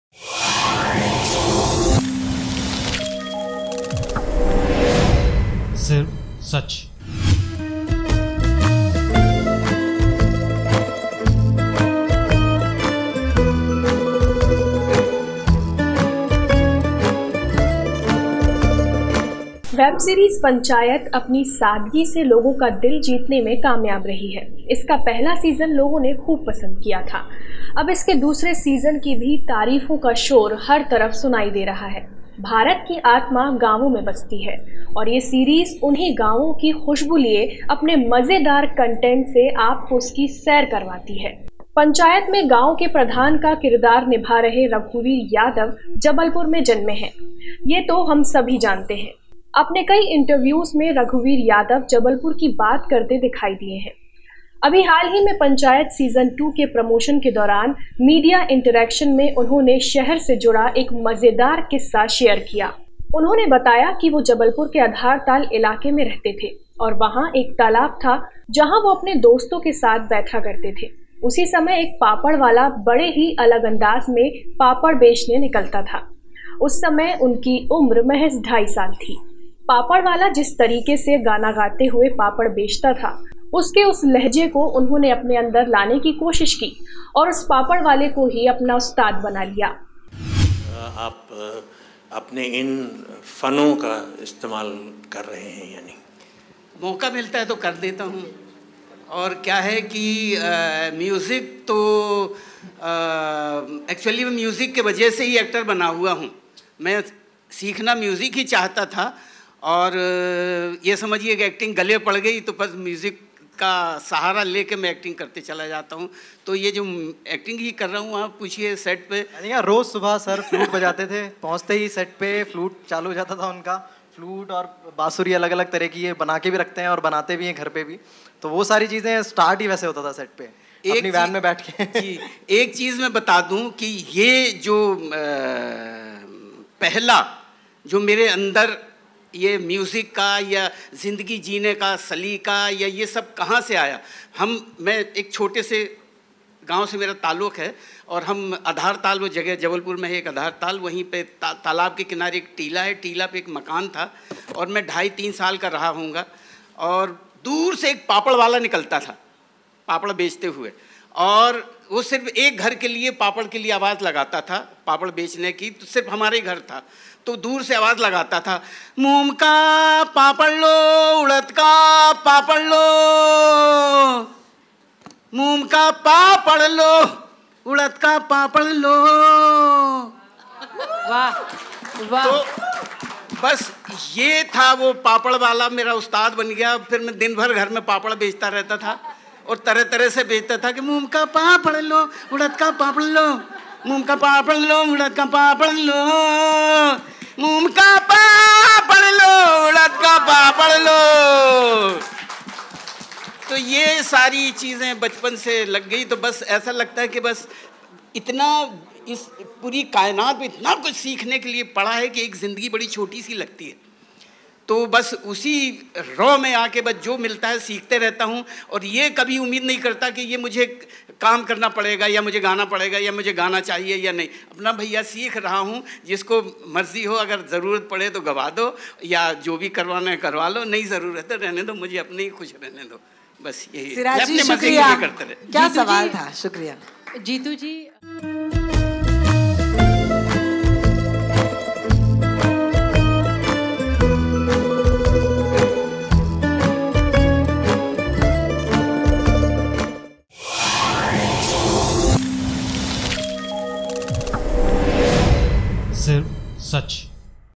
PANCHAYAT SEASON-2 के प्रमोशन के दौरान मीडिया से बातचीत में रघुवीर यादव ने JABALPUR को किया याद
पंचायत में गाँव के प्रधान का किरदार निभा रहे रघुवीर यादव जबलपुर में जन्मे है। अपने कई इंटरव्यूज में रघुवीर यादव जबलपुर की बात करते दिखाई दिए है। अभी हाल ही में पंचायत सीजन 2 के प्रमोशन के दौरान मीडिया इंटरैक्शन में उन्होंने शहर से जुड़ा एक मजेदार किस्सा शेयर किया।